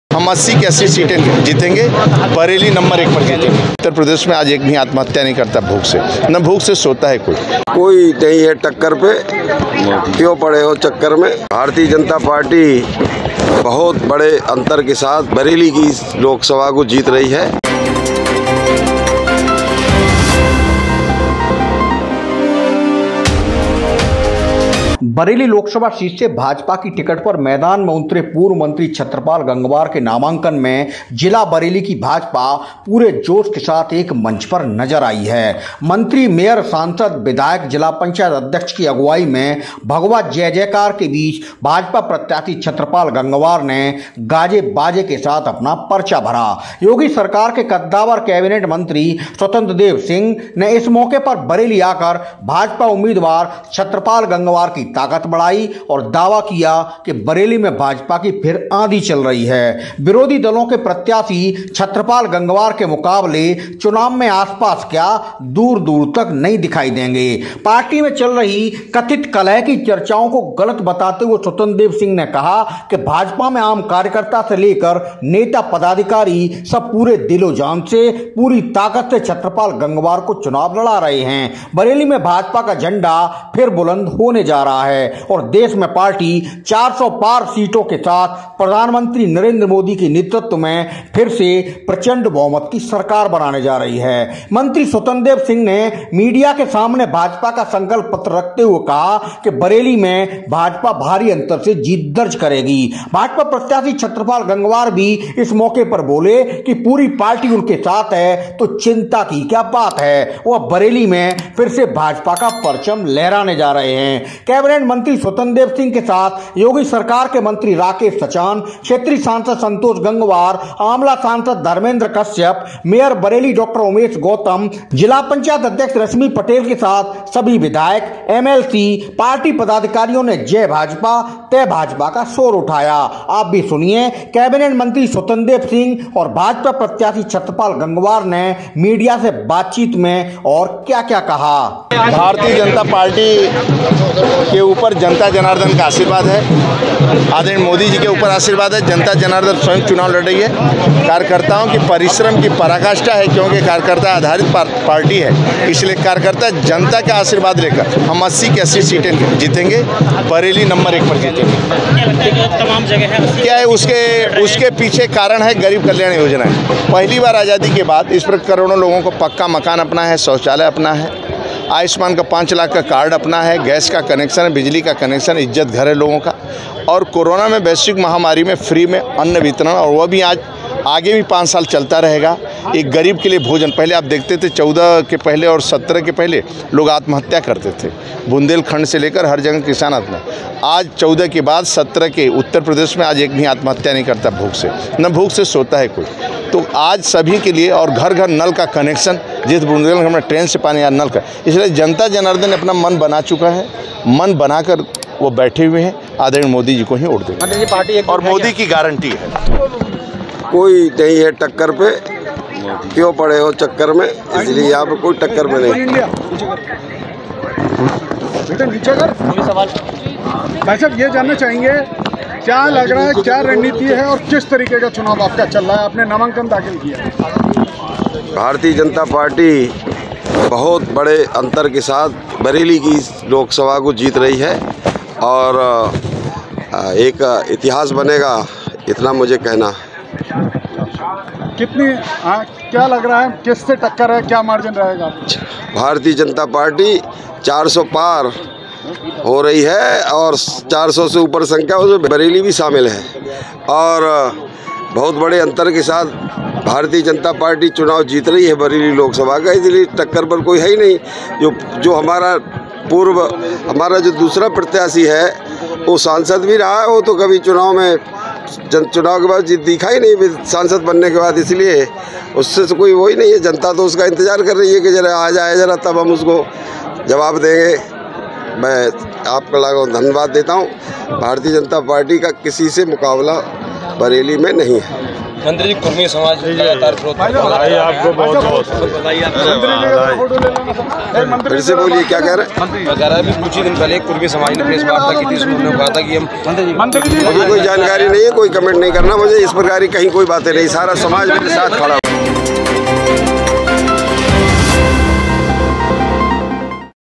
बोल बरेली: भाजपा प्रत्याशी छत्रपाल के नामांकन में सब साथ-साथ, लीडर बोले-चिंता की क्या बात
Welcome to channel khabarchee News channel

बरेली से भाजपा प्रत्याशी छत्रपाल ने नामांकन में दिखाई ताकत
छत्रपाल के समर्थन में कैबिनेट मंत्री स्वतंत्र देव सिंह ने की सभा
नामांकन में छत्रपाल के साथ दिखी पूरे बरेली जिले की भाजपा
सांसद संतोष गंगवार, मेयर उमेश गौतम ने बढ़ाया कार्यकर्ताओं में जोश
स्वतंत्र देव बोले, भाजपा में सब मिलकर बढ़ाते हैं कमल की ताकत
कलह की चर्चाओं पर बोले स्वतंत्र देव, बरेली में सब साथ-साथ
स्वतंत्र देव ने मीडिया के सामने दोहराया भाजपा का संकल्प पत्र
पीएम मोदी के नेतृत्व में देश का बज रहा दुनिया में डंका-स्वतंत्र देव
पीएम मोदी को समाज के हर वर्ग तबके के हितों का ख्याल- स्वतंत्र देव
भाजपा प्रत्याशी छत्रपाल ने कहा, बरेली में फिर बुलंद होगा भाजपा का झंडा
आंवला सांसद धर्मेन्द्र कश्यप ने दल-बल के साथ किया नामांकन
सांसद कश्यप की ताकत बढ़ाने पहुंचे डिप्टी सीएम केशव मौर्या
केशव प्रसाद ने भाजपा प्रत्याशी कश्यप के समर्थन में की सभा
केशव प्रसाद बोले, भाजपा में पहले कार्यकर्ता, फिर कोई और
यूपी में 80 की 80 सीटें जीत रही भाजपा-केशव प्रसाद मौर्या
आंवला सीट भाजपा की है और भाजपा की रहेगी-केशव मौर्या
समाजवादी पार्टी गुंडे-अपराधी करने वाली फैक्ट्री--केशव मौर्या
भाजपा के संकल्प पत्र में हर वर्ग तबके का हित-केशव मौर्या
पीएम मोदी के नेतृत्व में विकसित देश बनेगा भारत-केशव मौर्या
मोदी हैं तो भरोसा है, बाकी सब धोखा है, बोले केशव प्रसाद मौर्या